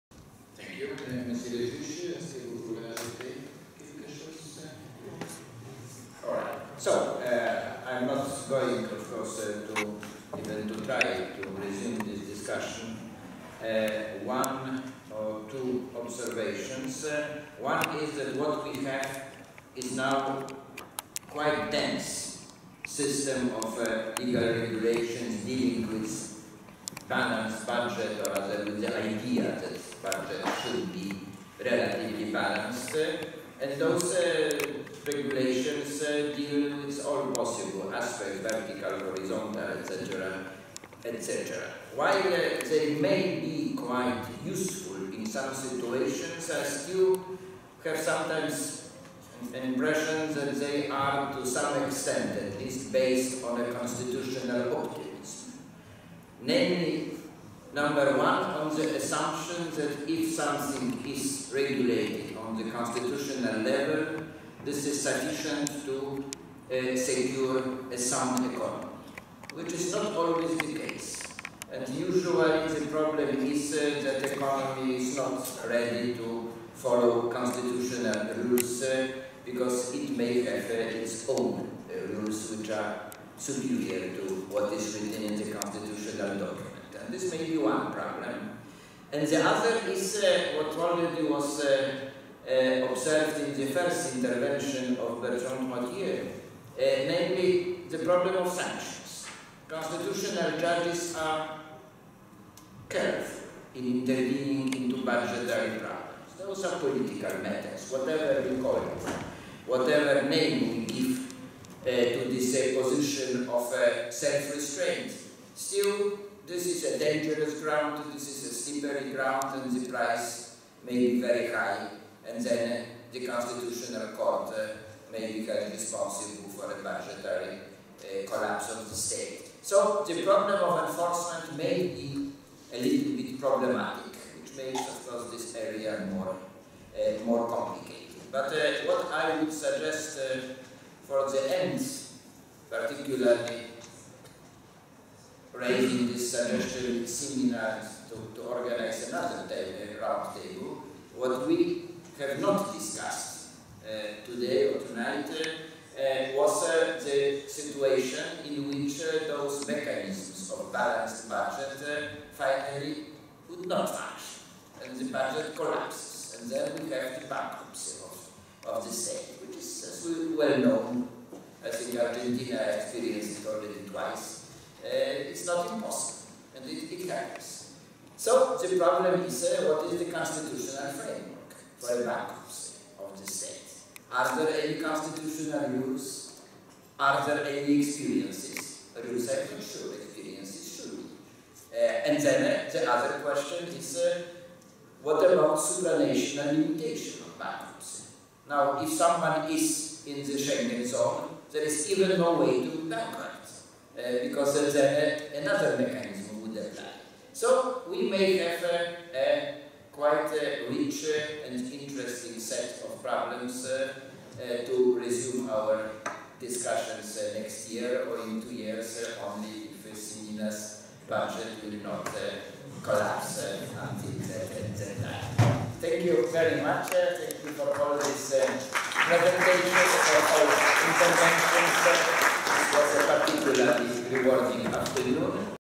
Lech GARLICKI: Final speech Shaping Constitutional Rules for a Balanced Budget
International Conference Shaping Constitutional Rules for a Balanced Budget
Faculty of Law, University of Bucharest, Romania
21 November 2014
14:00-16:30
II. Balanced Budget Rule and Separation of Powers: Chair Lech Garlicki (France)